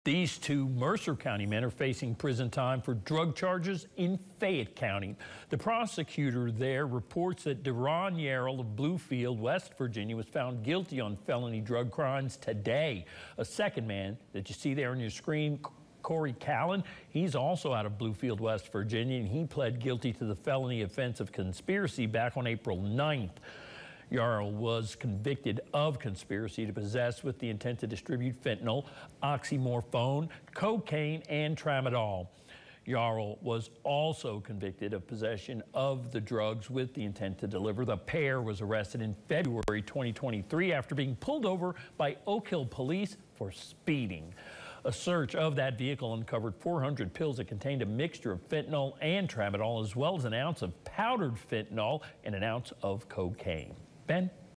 Two face prison time for drug charges in Fayette County
Two men are facing prison time for drug charges in Fayette County.